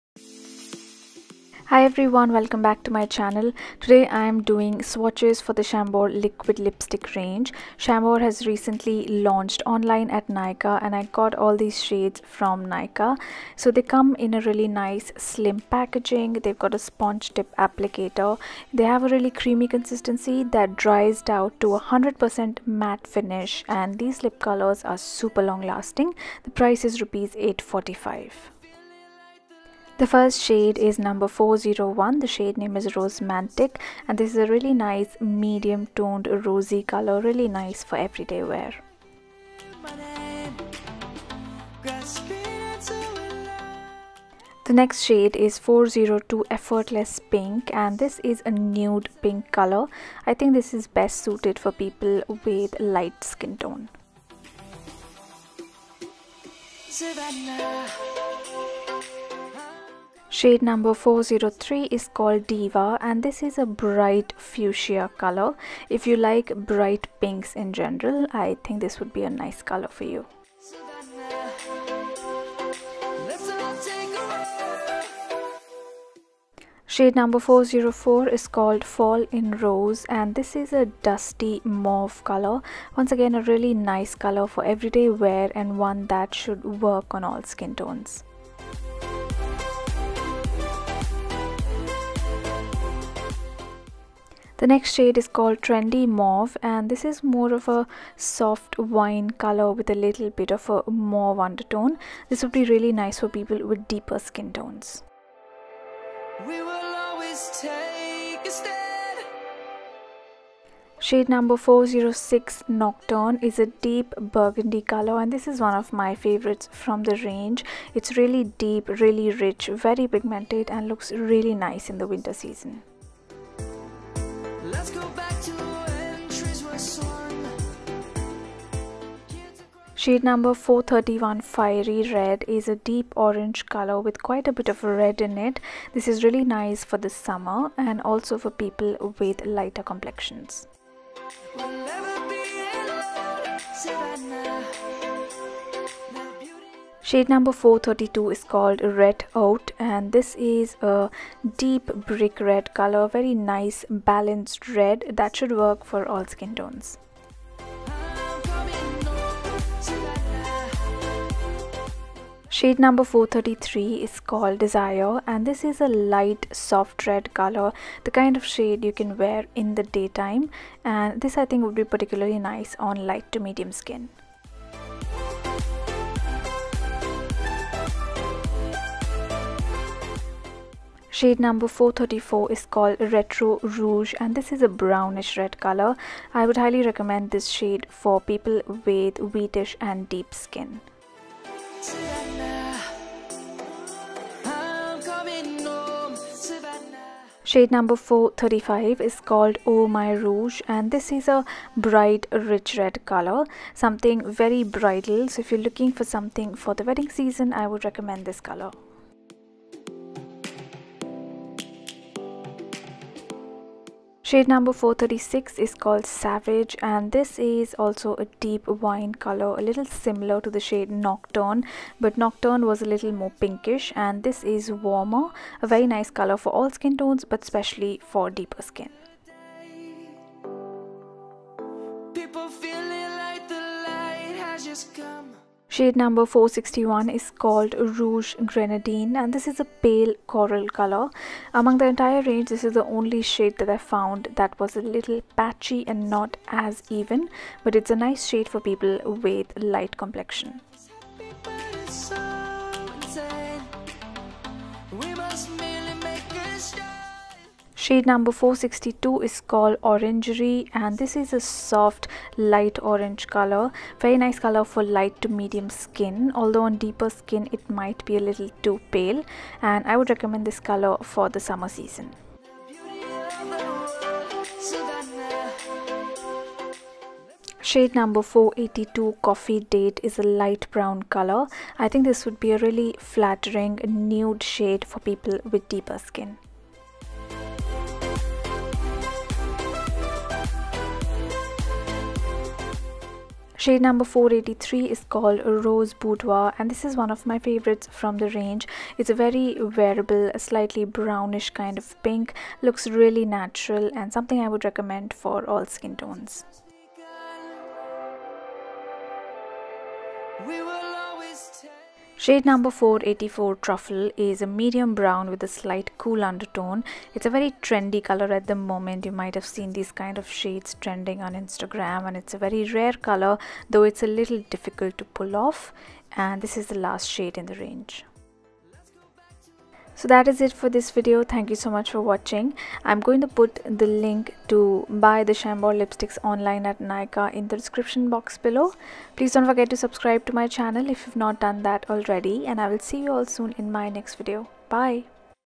SWATCHES 💄 Chambor Extreme Wear Transferproof Liquid Lipstick | corallista
Chambor Extreme Wear Transferproof Liquid Lipstick SWATCHES!

Chambor products are now available online at Nykaa.com.

Chambor Liquid Lipstick | Price - Rs 845
Available in 18 shades.

Order of swatches :

401 Rosemantic
402 Effortless Pink
403 Diva
404 Fall in Rose
405 Trendy Mauve
406 Nocturne

431 Fiery Red
432 Red Haute
433 Desire
434 Retro Rouge
435 Oh My Rouge
436 Savage

461 Rouge Grenadine
462 Orangerie
482 Coffee Date
483 Rose Boudoir
484 Truffle

Camera I use for my videos and pictures : Canon 80D DSLR

Body only
EF-S 18-55mm
EF-S 18-135mm